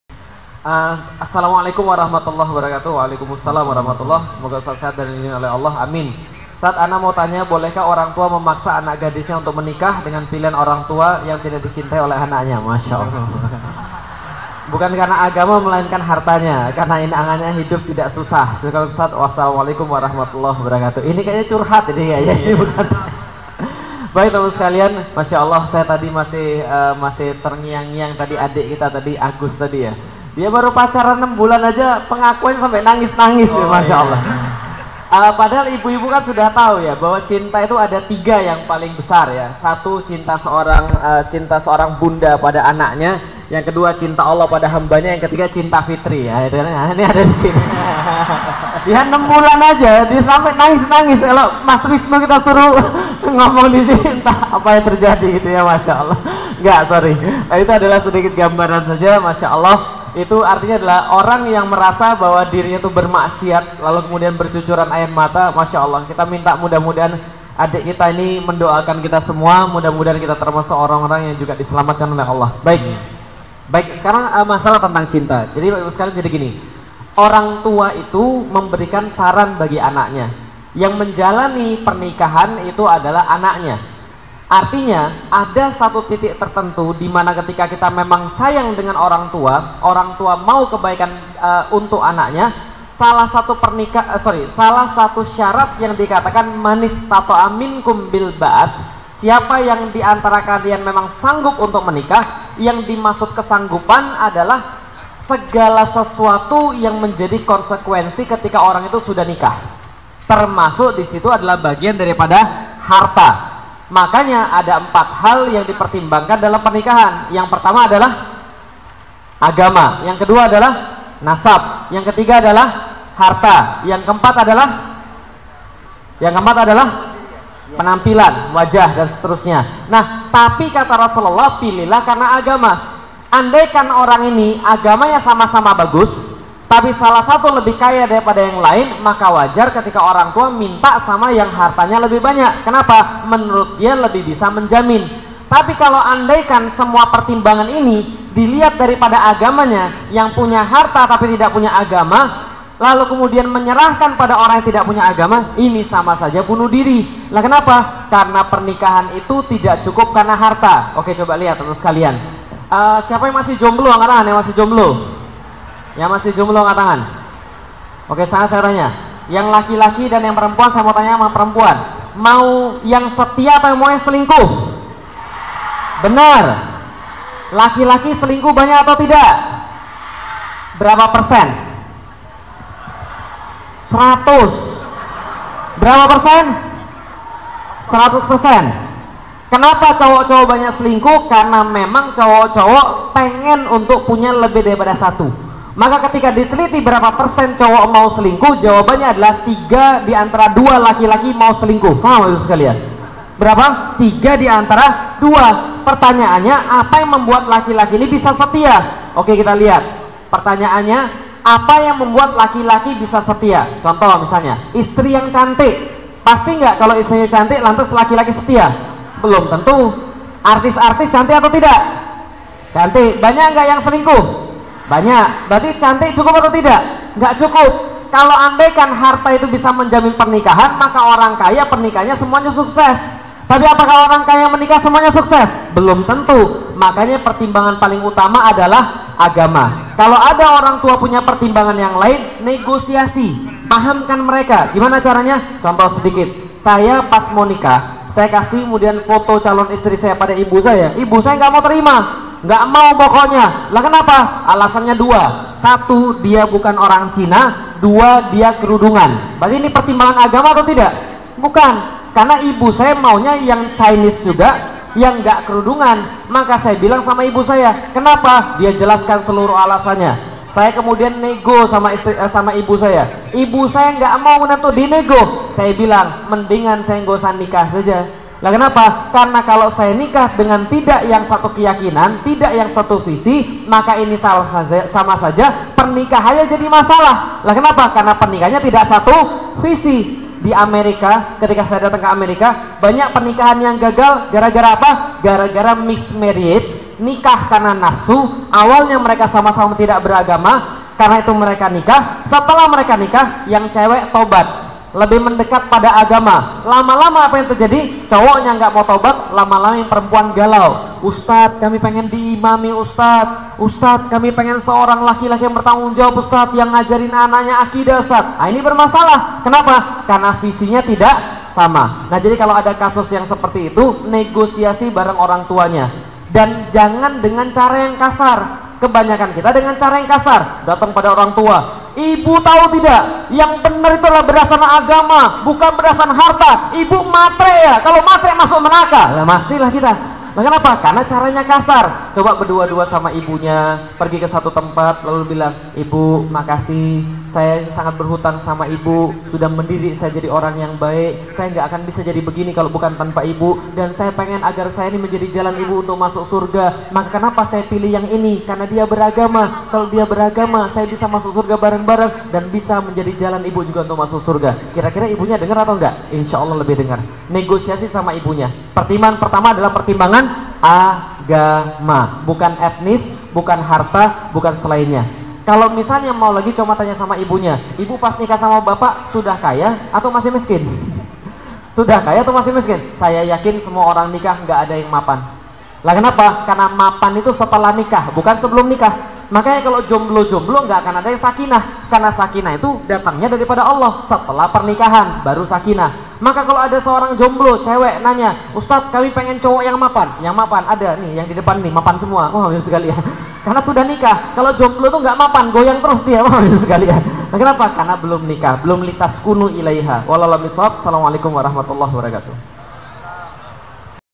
Solusi Bagi seseorang yang di paksa menikah oleh orang tua - Ustadz Felix Siauw
Bagi seseorang yang sedang atau mau di nikahkan orang tua karena menurut orang tua dia lebih kaya. tapi dia bukanlah orang yang kau cinta